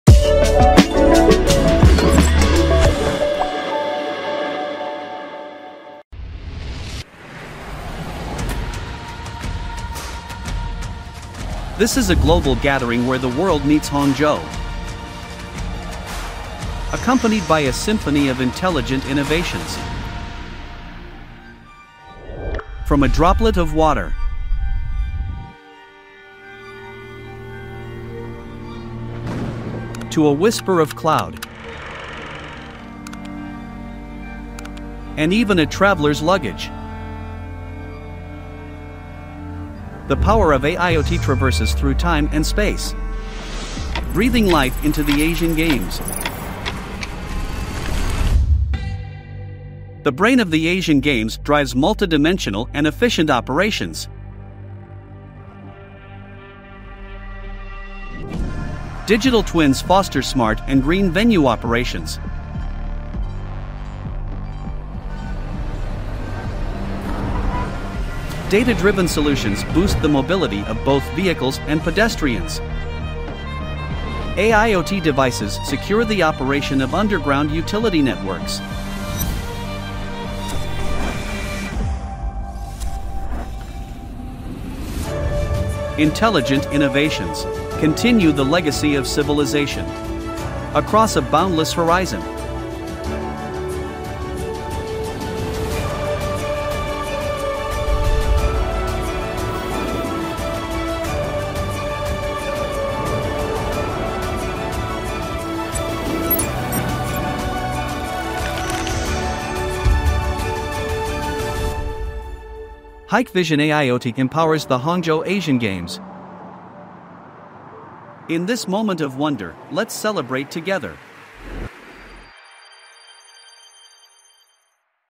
AIoT Empowers the Asian Games
The power of AIoT traverses through time and space, breathing life into the Asian Games.
Hikvision AIoT empowers the Hangzhou Asian Games.
In this moment of wonder, let’s celebrate together.